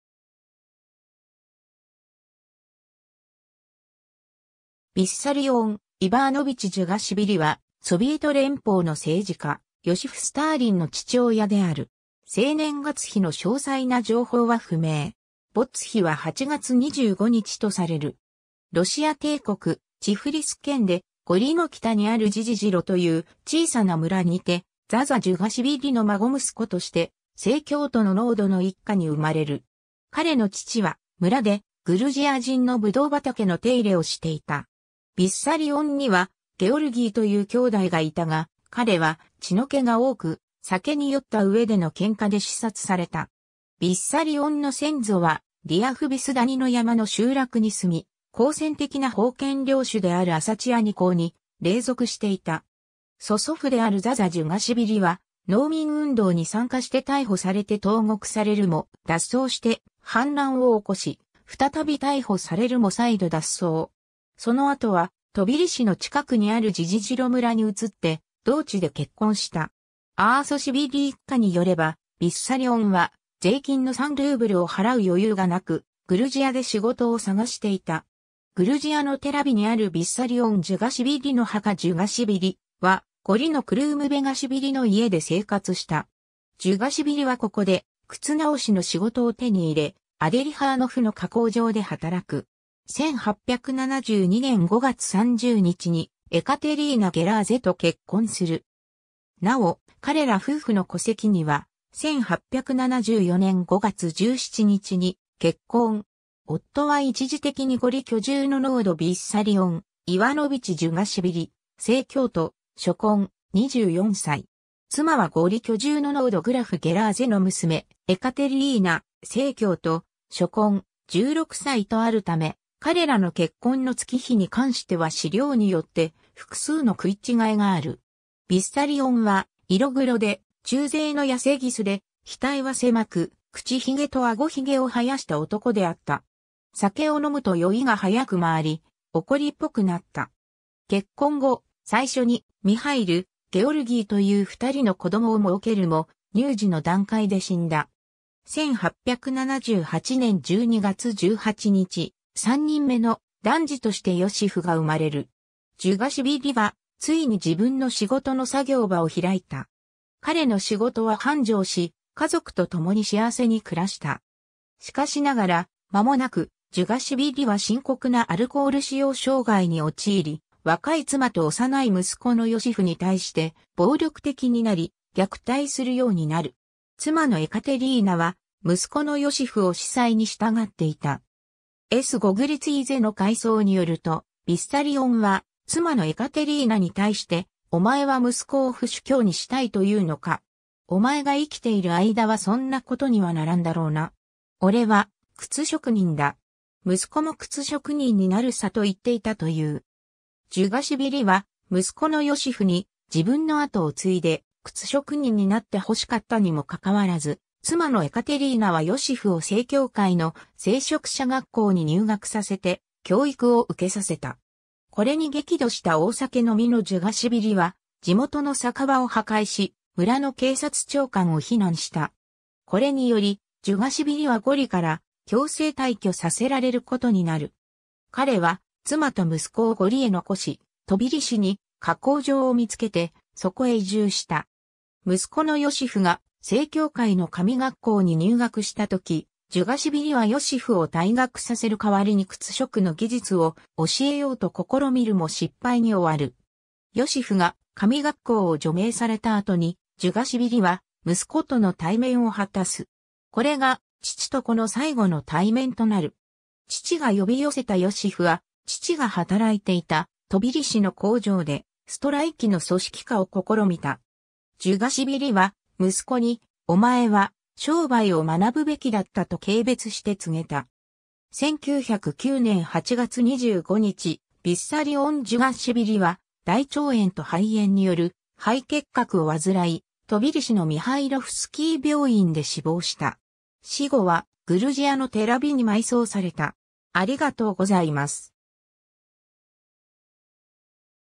ヴィッサリオン・ジュガシヴィリ
ヨシフ・スターリン
ヴィッサリオン（ベソ）・イヴァーノヴィチ・ジュガシヴィリ（ロシア語: Виссарион (Бесо) Иванович Джугашвили、グルジア語: ბესარიონ ივანეს ძე ჯუღაშვილი、1850年頃 - 1909年）は、ソビエト連邦の政治家ヨシフ・スターリンの父親である。
生年月日の詳細な情報は不明。
没日は8月25日とされる。
 ロシア帝国チフリス県で、ゴリの北にあるジジ・ジロ(ka)という小さな村にて、ザザ・ジュガシヴィリの孫息子として、正教徒の農奴の一家に生まれる。
彼の父は、村でグルジア人の葡萄畑の手入れをしていた。
ヴィッサリオンにはゲオルギーという兄弟がいたが、彼は血の気が多く、酒に酔ったうえでの喧嘩で刺殺された。
ヴィッサリオンの先祖はリアフヴィス谷の山の集落に住み、好戦的な封建領主であるアサチアニ公に隷属していた。
曽祖父であるザザ・ジュガシヴィリは、農民運動に参加して逮捕されて投獄されるも脱走して反乱を起こし、再び逮捕されるも再度脱走。
その後はトビリシの近くにあるジジ・ジロ村に移って同地で結婚した。
 アーソシヴィリ一家（ヴィサッリオンの親戚で、長い間村に住んでいた）によれば、ヴィッサリオンは税金の3ルーブルを払う余裕がなく、グルジアで仕事を探していた。
 グルジアのテラビにあるヴィッサリオン・ジュガシヴィリの墓 ジュガシヴィリは、ゴリのクルームベガシヴィリの家で生活した。
ジュガシヴィリはここで靴直しの仕事を手に入れ、アデリハーノフの靴工場で働く。
1872年5月30日にエカテリーナ・ゲラーゼと結婚する。
なお、彼ら夫婦の戸籍には「1874年5月17日に結婚。夫は一時的にゴリ居住の農奴ヴィッサリオン・イワノヴィチ・ジュガシヴィリ、正教徒、初婚、24歳。妻はゴリ居住の農奴グラフ・ゲラーゼの娘、エカテリーナ、正教徒、初婚、16歳」とあるため、彼らの結婚の月日に関しては資料によって複数の食い違いがある。
 ヴィッサリオンは、色黒で、中背のやせぎすで、額は狭く、口ひげと顎ひげを生やした男であった。
酒を飲むと酔いが早く回り、怒りっぽくなった。
結婚後、最初にミハイル、ゲオルギーという2人の子供を儲けるも、乳児の段階で死んだ。
1878年12月18日、3人目の男児としてヨシフが生まれる。
 ジュガシヴィリはついに自分の仕事の作業場を開いた。
彼の仕事は繁盛し、家族とともに幸せに暮らした。
しかしながら、まもなくジュガシヴィリは深刻なアルコール使用障害に陥り、若い妻と幼い息子のヨシフに対して暴力的になり、虐待するようになる。
 妻のエカテリーナは、息子のヨシフを司祭にしたがっていた。
S・ゴグリツィーゼの回想によると、ヴィッサリオンは妻のエカテリーナに対して「お前は息子を府主教にしたいというのか？お前が生きている間はそんなことにはならんだろうな。おれは靴職人だ。息子も靴職人になるさ」と言っていたという。
ジュガシヴィリは、息子のヨシフに自分の跡を継いで靴職人になって欲しかったにもかかわらず、妻のエカテリーナはヨシフを正教会の聖職者学校に入学させて教育を受けさせた。
 これに激怒した大酒飲みのジュガシヴィリは、地元の酒場を破壊し、村の警察長官を非難した。
これにより、ジュガシヴィリはゴリから強制退去させられることになる。
彼は妻と息子をゴリへ残し、トビリシに靴工場を見つけてそこへ移住した。
 息子のヨシフが正教会の神学校に入学したとき、ジュガシヴィリはヨシフを退学させる代わりに靴職の技術を教えようと試みるも失敗に終わる。
ヨシフが神学校を除名されたあとに、ジュガシヴィリは息子との対面を果たす。
これが父と子の最後の対面となる。
父が呼び寄せたヨシフは、父が働いていたトビリシの工場でストライキの組織化を試みた。
 ジュガシヴィリは息子に、「お前は商売を学ぶべきだった」と軽蔑して告げた。
 1909年8月25日、ヴィッサリオン・ジュガシヴィリは、大腸炎と肺炎による肺結核を患い、トビリシのミハイロフスキー病院で死亡した。
 死後はグルジア（現在のジョージア国）のテラヴィに埋葬された。